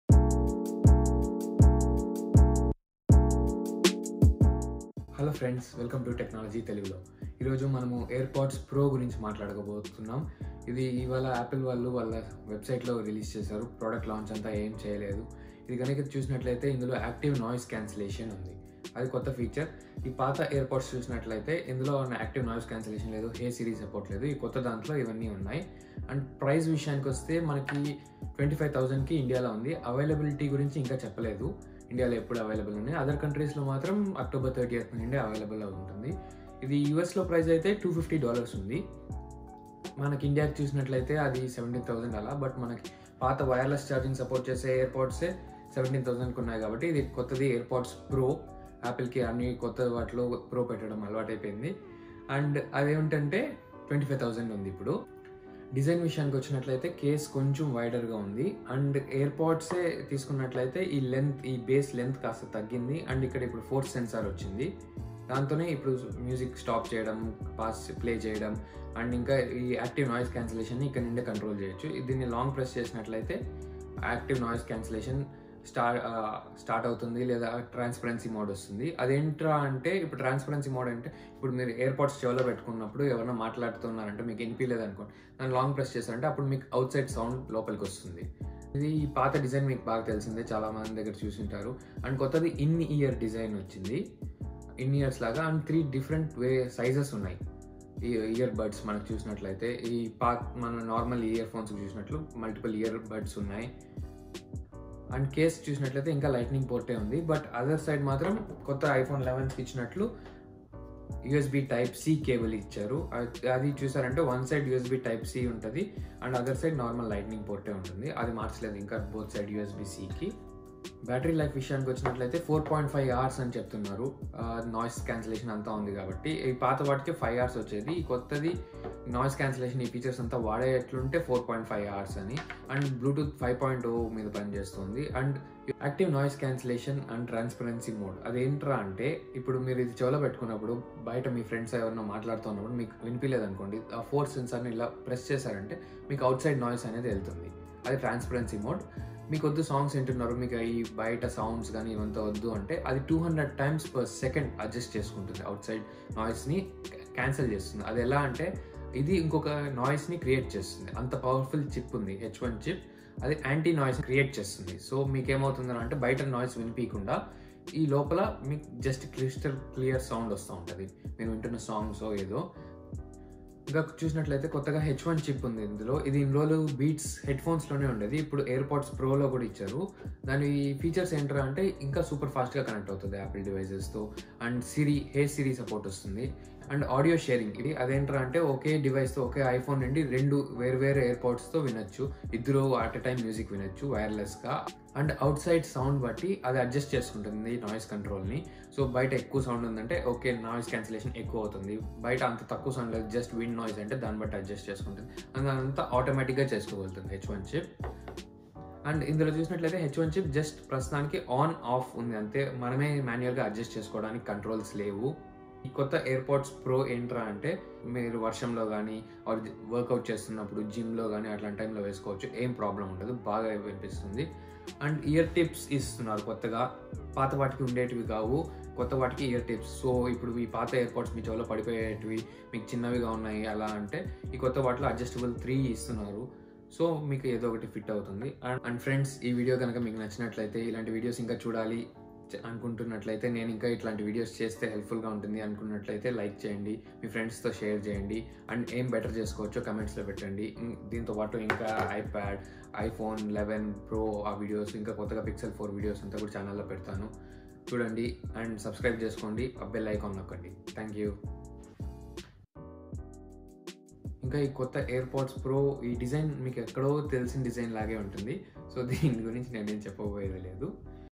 AirPods Pro | Apple's 2019 AirPods in Telugu
AirPods Pro have been announced! They're slightly redesigned, smaller and come with Noise Cancellation! Here are my thoughts and first impressions.

The price in India is Rs.24900 and are not available yet. The available date of these new AirPods Pro will be announced soon.

New features of AirPods Pro are described in the video and are compared with old Apple AirPods.

To check prices and buy the product, follow the links:
Apple AirPods with Wireless Charging Case
Apple AirPods with Charging Case